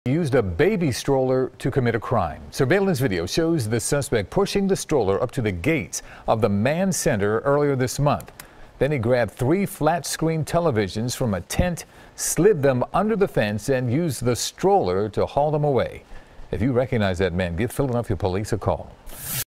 Thief Uses Baby Stroller To Steal TV's
This happened at the Mann Center earlier in July.